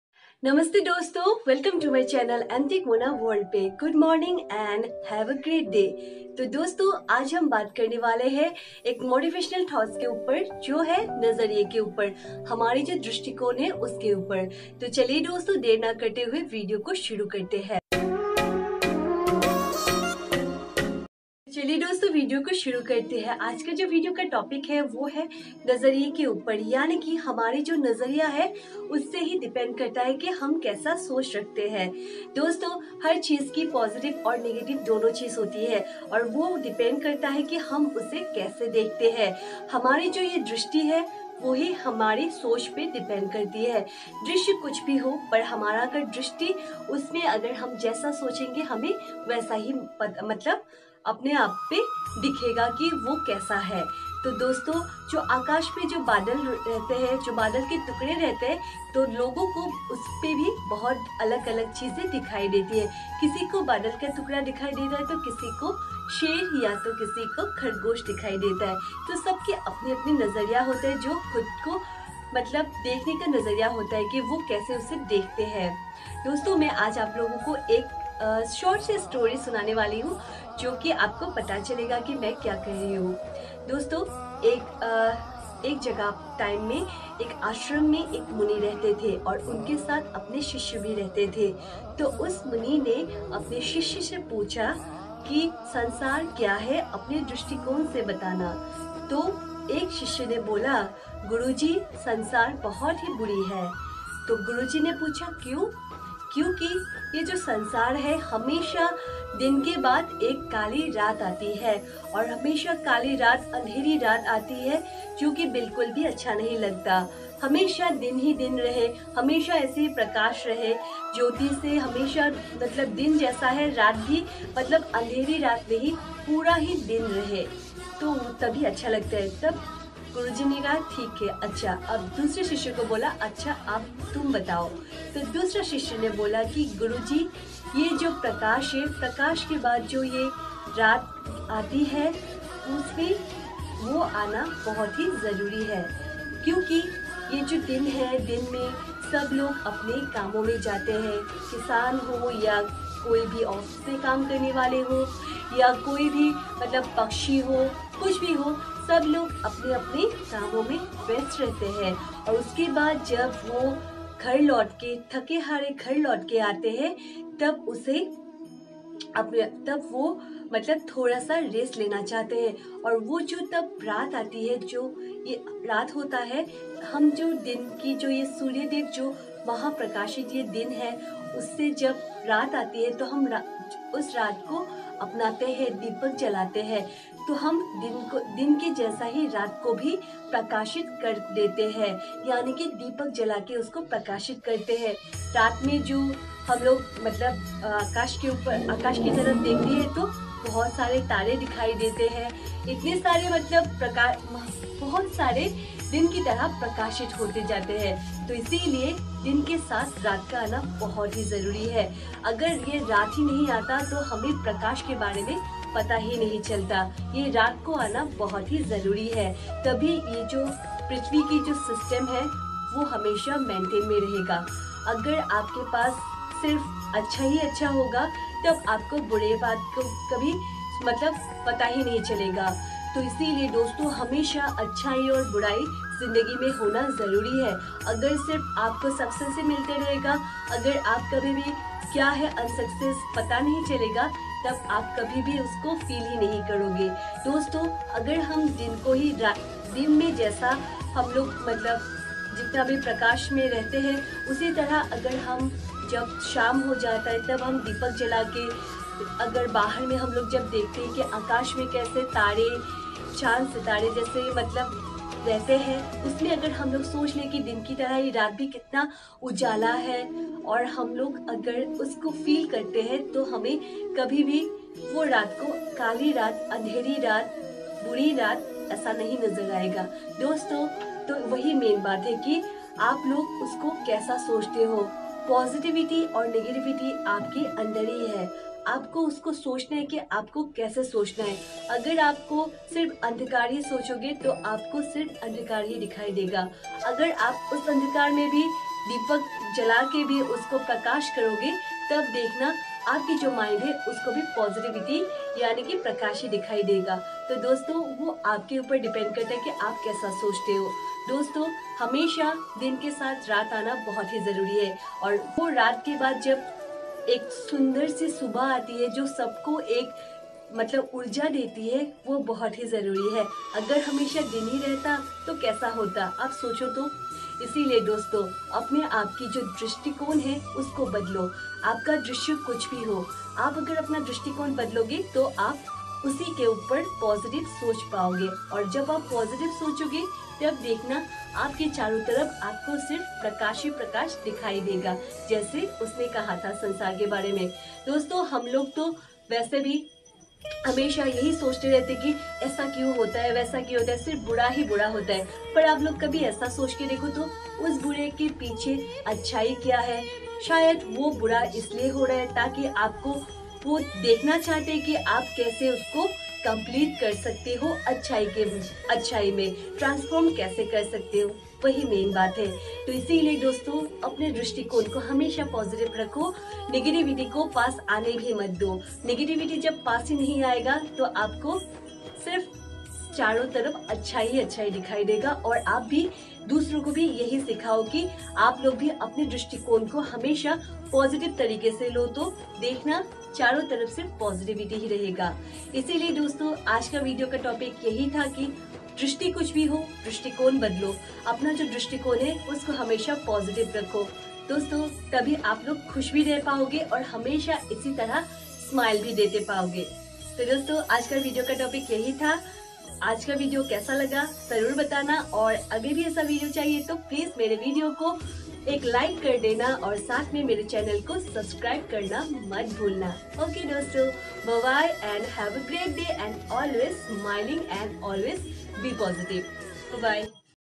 The Power Of Positive Thinking Motivational video in Hindi
Is video me mene apne bataya he ki apna ji najariya he usko hum jese dekhna chahte he wese hi dekhte he.. Postive sochoge tabhi aapko positively nazar aayenge... Aaj ka topic isi ko bataya he aaplog dekho aur batana mat bhulna aaplogo ko kesa laga mera video aur sath me like vi kar dena n sath me Comment karna mat bhulna 😊

THANKING you 😘